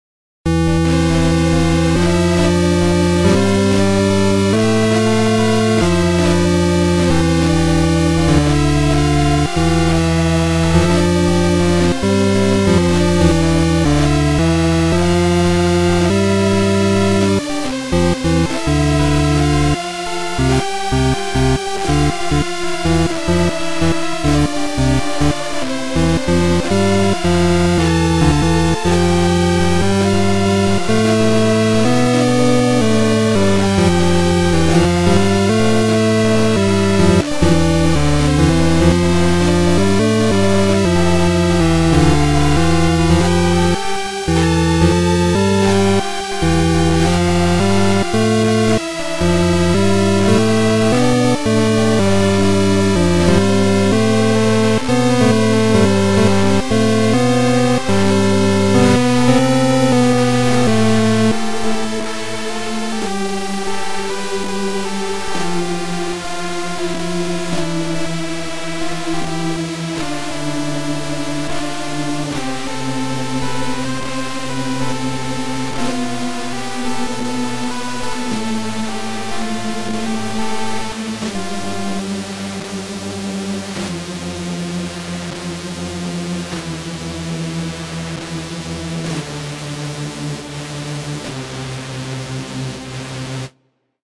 is this all you have
imagen out of nowhere being given a very short amount of time to make the photo for this this is the reasult so ya wouldve possibley been better but nope couldent cuz of a unknown and uneeded dead line so enjoy it if you do cuz i cant enjoy it at all rn